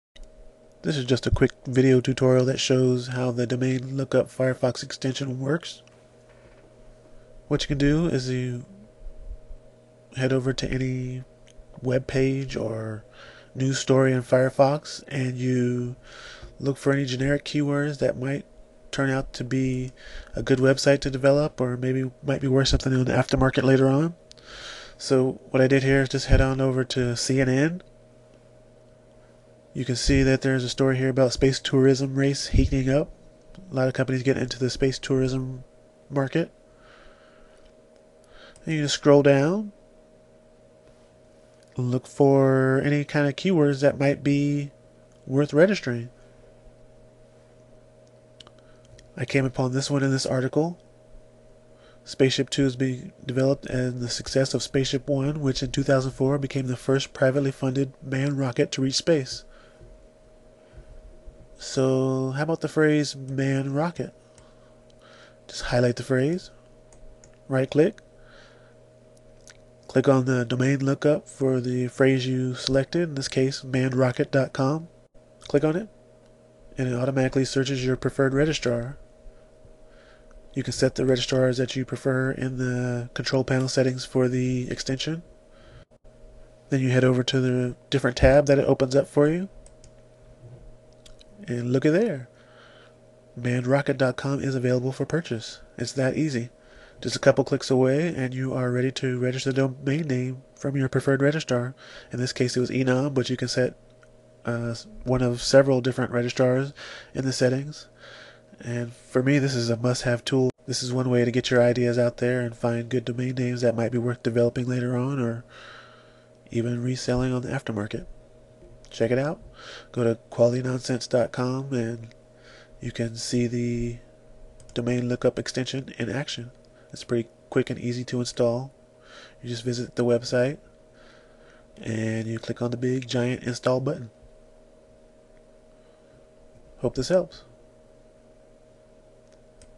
domainer tool - domain lookup firefox extension
A quickie video that shows the cool the Domain Lookup Firefox Extension available from QualityNonsense.

I didn't create this tool, I'm just a domainjunkie who heard about it and uses it daily :)